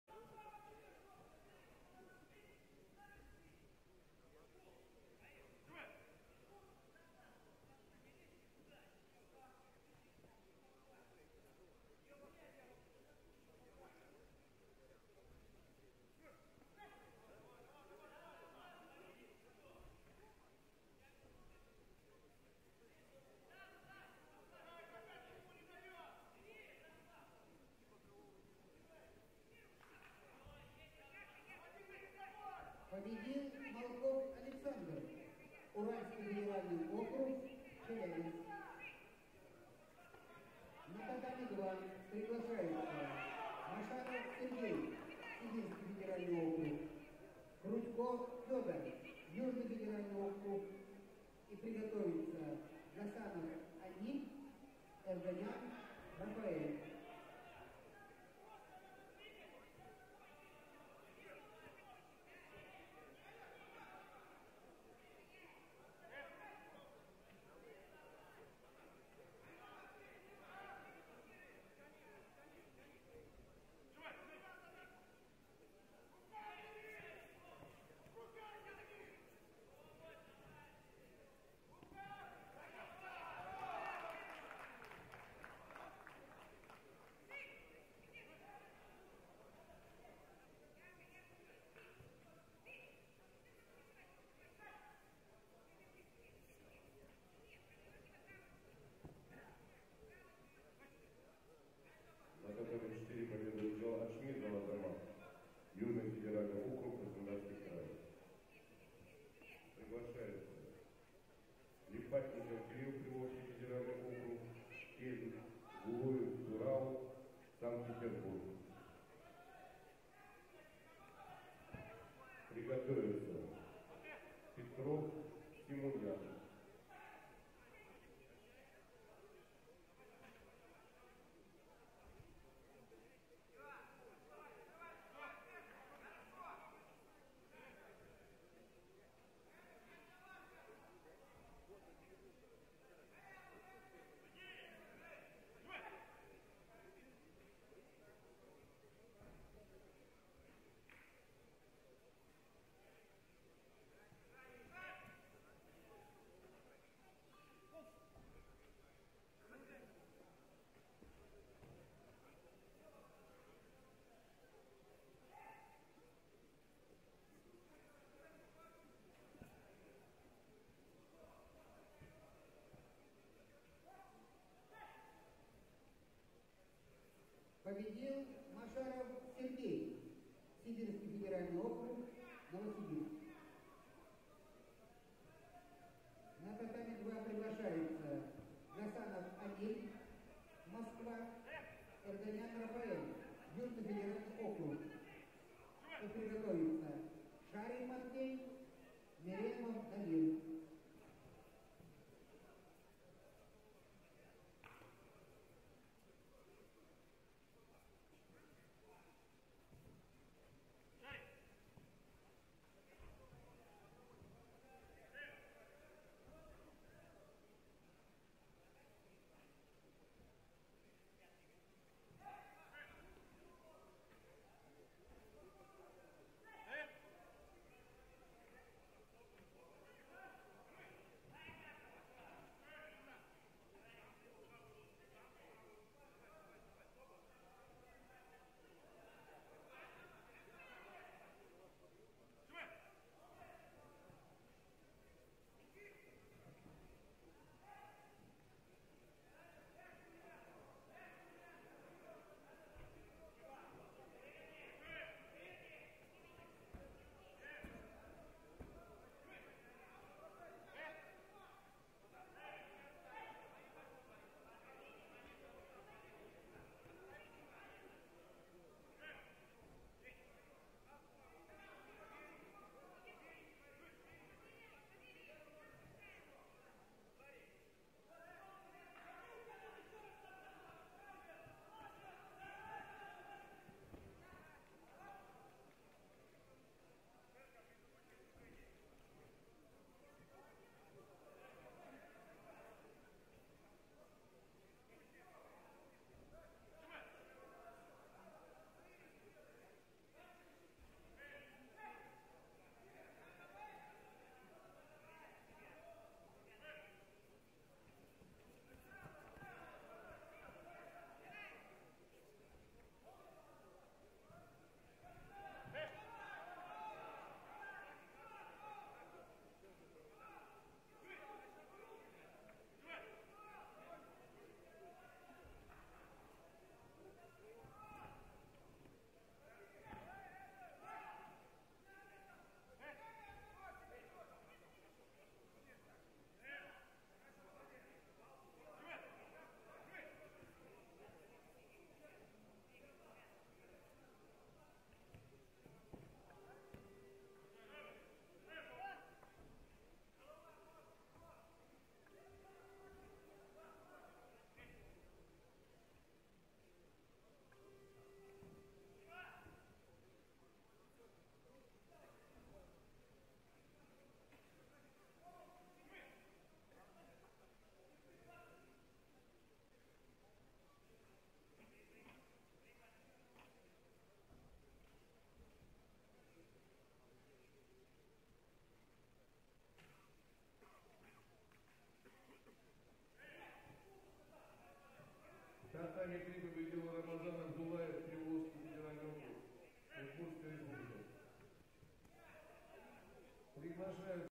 60 kg Абдулаев Р Гаджиев С 05 03 2016 Т3 ПЕРВЕНСТВО РОССИИ до 21, РОСТОВ НА ДОНУ 2016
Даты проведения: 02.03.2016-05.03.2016
Место проведения: Россия, Ростов-на Дону
Возрастная категория: юниоры и юниорки 1996-98 г.р.